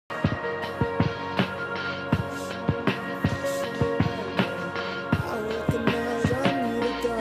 shorts vloggear backontrack | TheOKChannel
because the phone tripod had broken and i couldnt get time to buy earlier and shoot new video. so here i am back on track 🙃